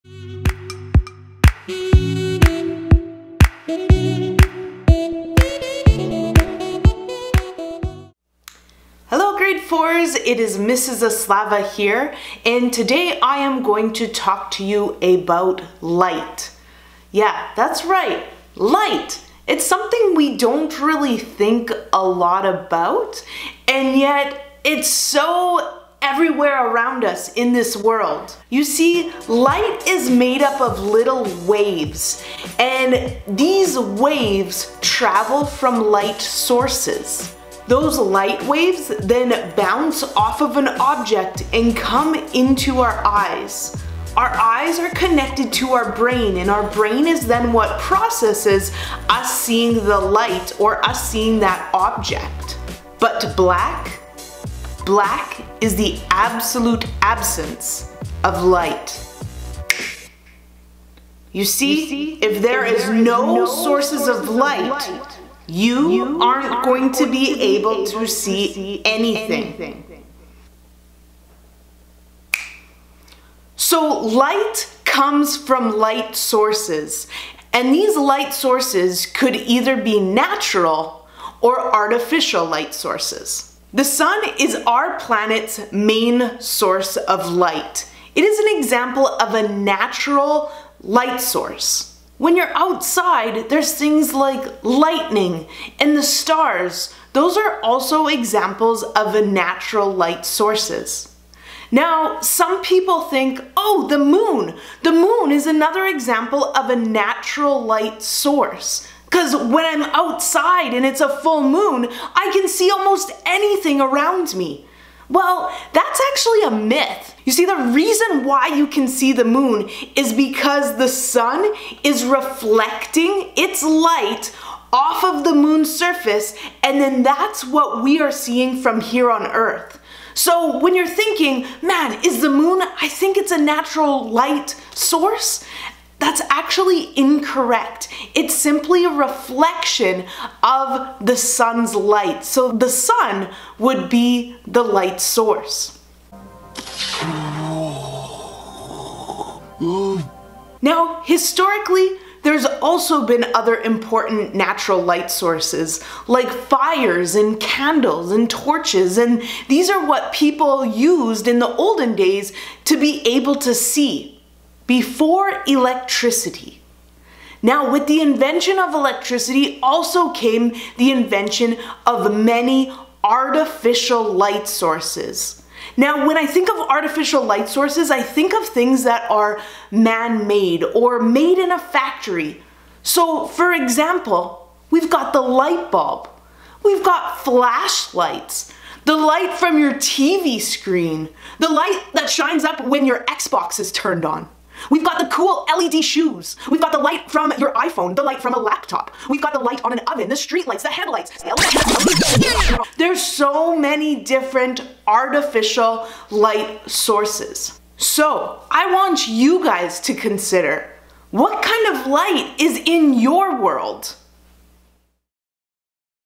Light Sources - Artificial Vs. Natural Light
This video explains the difference between natural and artificial light sources. It also provides many examples of each type of light source.